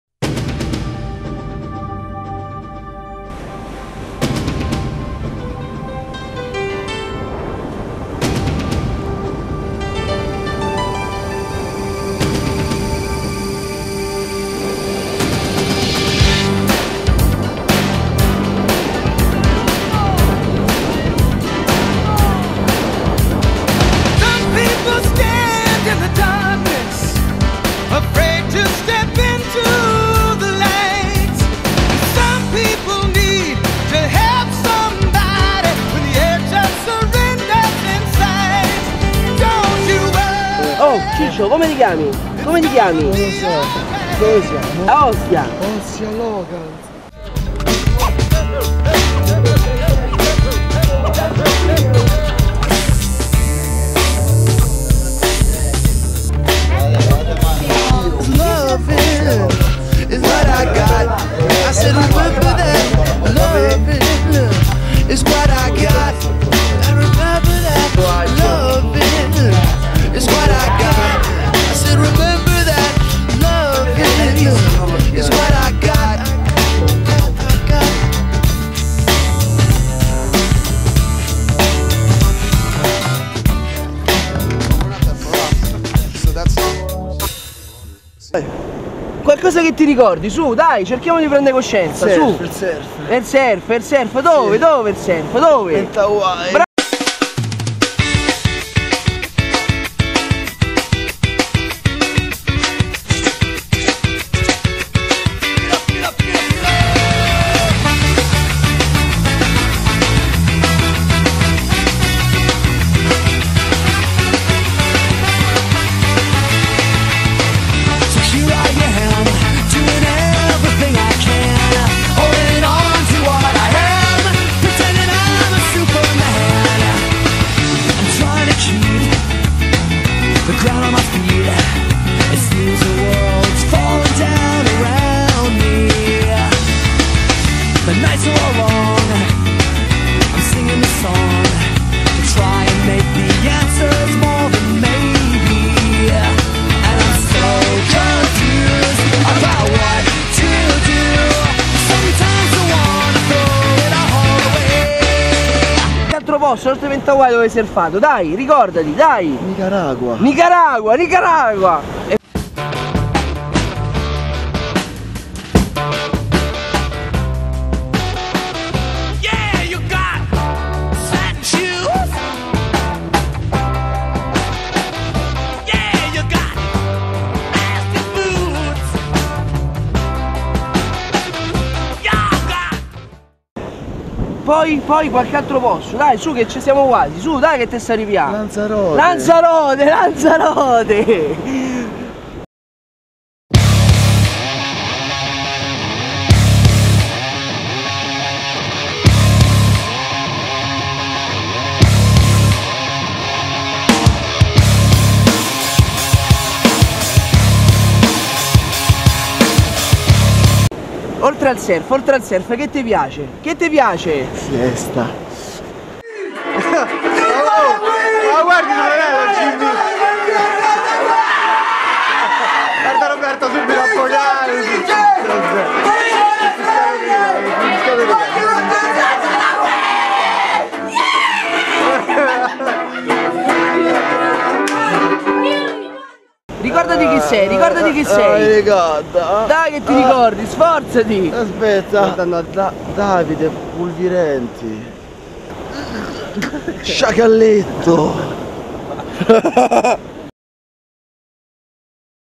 GloryTricks2012 - LO SMEMORATO
Video-Concorso GloryTricks 2012

Rider :Davide Pulvirenti
Bagnino : Gianmaria Giuntoli
Passante : Gianmaria Giuntoli
Riprese : Gianmaria Giuntoli, Emanuele Mariotti, Davide Pulvirenti
Montaggio : Gianmaria Giuntoli